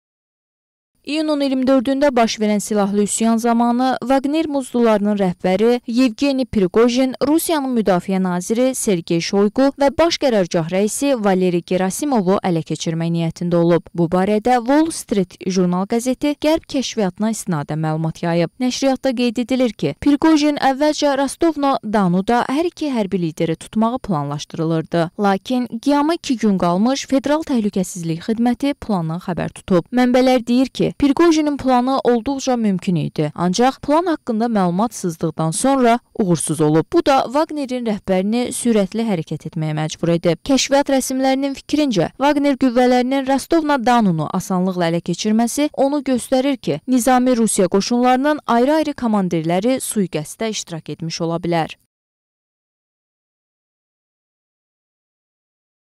Priqojinin ilginc planı niyə alınmadı? - APA TV
İyunun 24-də baş verən silahlı üsyan zamanı "Vaqner" muzdlularının rəhbəri Yevgeni Priqojin Rusiyanın müdafiə naziri Sergey Şoyqu və Baş Qərargah rəisi Valeri Gerasimovu ələ keçirmək niyyətində olub.

Bu barədə "Wall Street Journal" qəzeti Qərb kəşfiyyatına istinadən məlumat yayıb.

Nəşr qeyd edir ki, Priqojin əvvəlcə Rostov-na-Donda hər iki hərbi lideri tutmağı planlaşdırılırdı, lakin qiyama iki gün qalmış Federal Təhlükəsizlik Xidməti (FTX) plandan xəbər tutub.

Mənbələr deyir ki, Priqojinin planı olduqca mümkün idi, ancaq plan haqqında məlumat sızdıqdan sonra uğursuz olub, bu da "Vaqner"in rəhbərini sürətli hərəkət etməyə məcbur edib

Kəşfiyyat rəsmilərinin fikrincə, "Vaqner" qüvvələrinin Rostov-na-Donu asanlıqla ələ keçirməsi onu göstərir ki, nizami Rusiya qoşunlarının ayrı-ayrı komandirləri sui-qəsddə iştirak etmiş ola bilər.